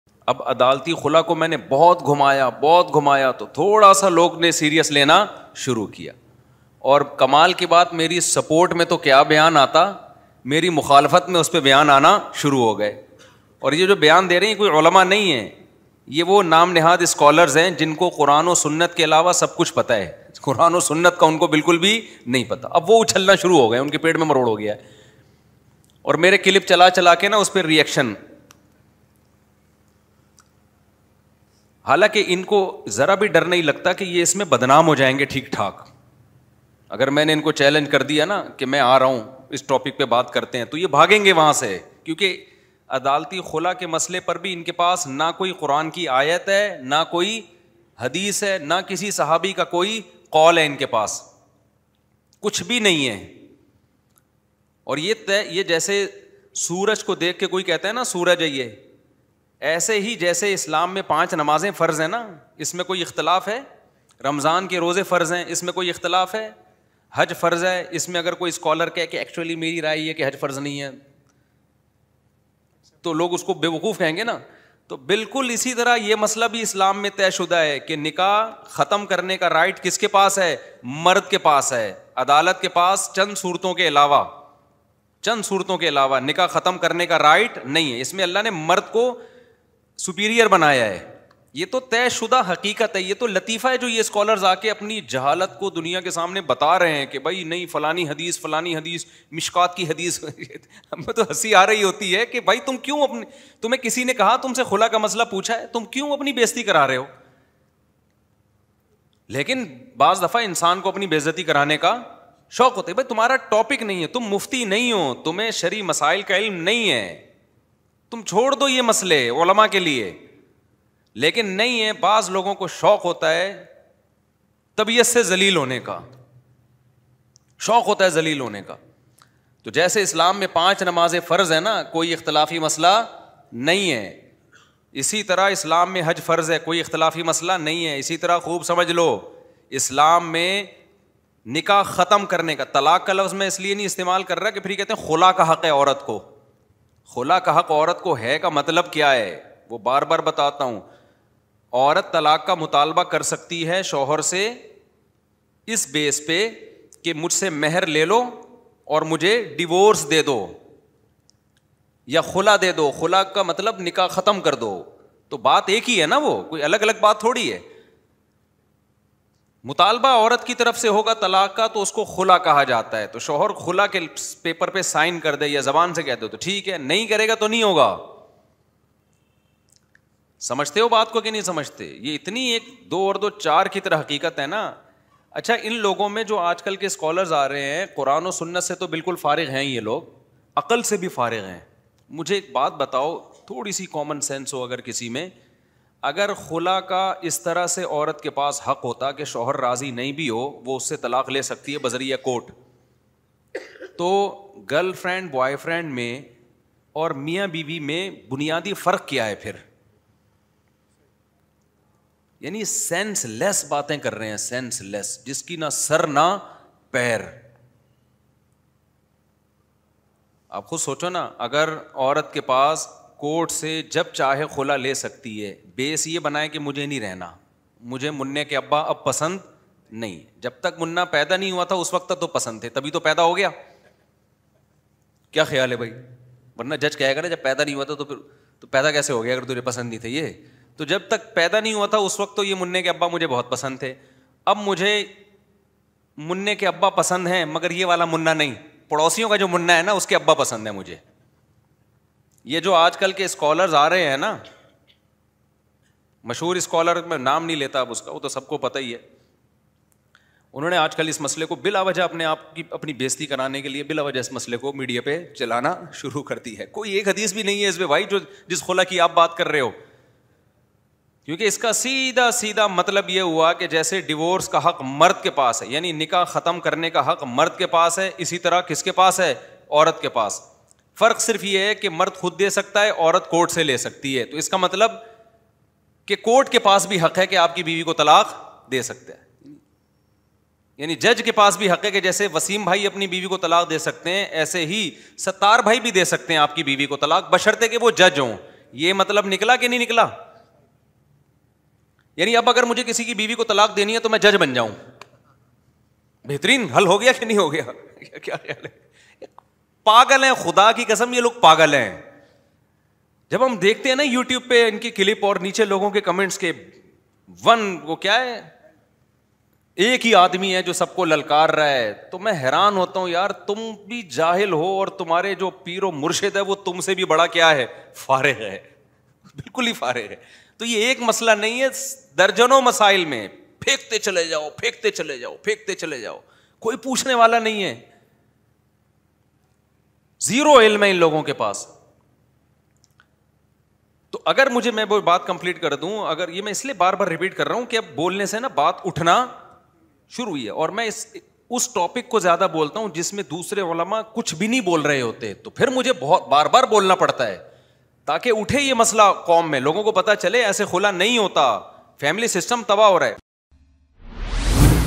Mufti Tariq Masood reply to Engineer Ali Mirza's latest Video! Adalti Khula ki haqeeqat kya hai ?
انجینئر محمد علی مرزا کا عدالتی خلا پر فضول تبصرہ اور مفتی طارق مسعود صاحب کا کرارا جواب